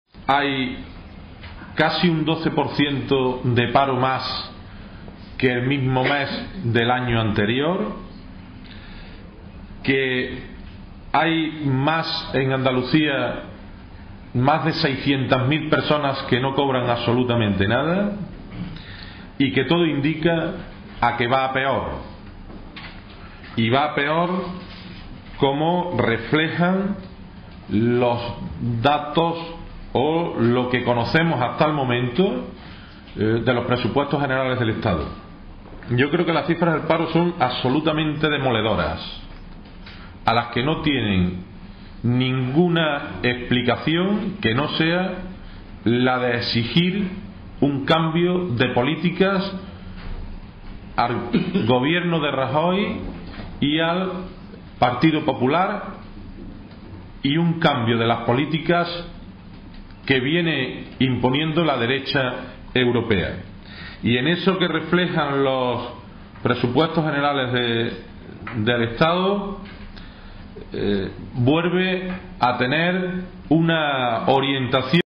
CCOO ve demoledoras las cifras del paro e invita a toda la ciudadanía al 7 de octubre
Carbonero ha calificado de "demoledoras" las cifras del paro que se han conocido y ha apostillado que "otra vez ponen de manifiesto la incapacidad de las medidas del gobierno para paliar la situación de pobreza en la que cada vez se encuentran más personas en este país. Solo en Andalucía ya hay más de 600.000 personas que no cobran nada".